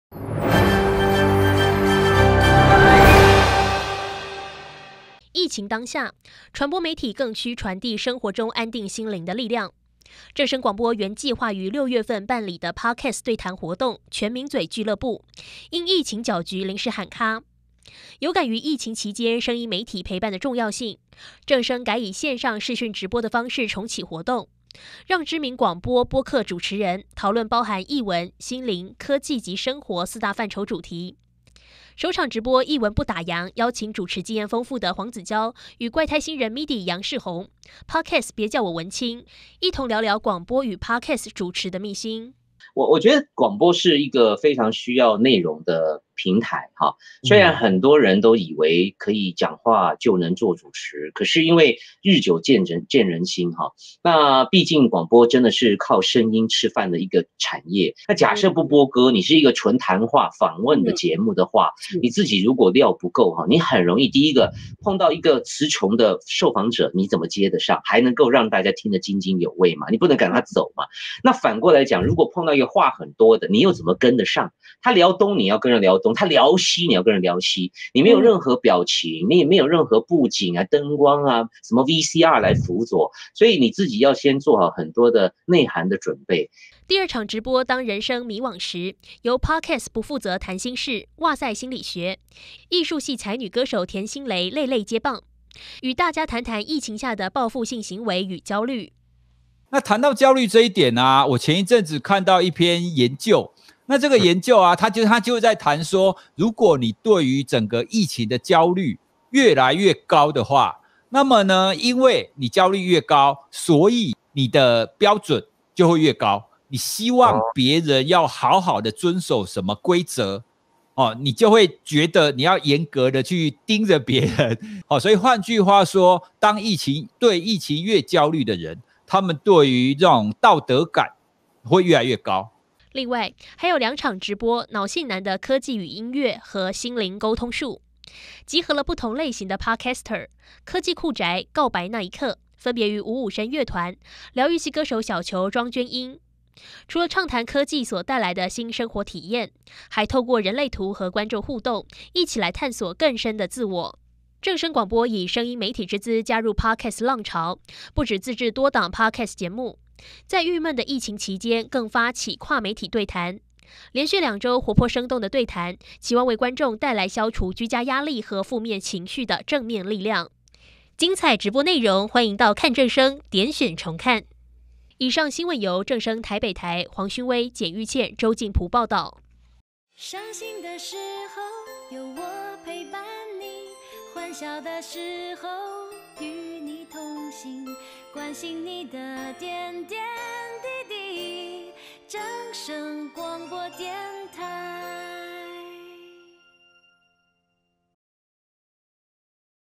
【CSNEWS】 全名嘴跨界對談 傳遞安心的力量
正聲廣播原計畫於6月份辦理的Podcast對談活動「全名嘴聚樂部」，因疫情攪局臨時喊卡。有感於疫情期間，聲音媒體陪伴的重要性，正聲改以線上視訊直播的方式重啟活動，讓大家在居家期間，也能有安心的力量。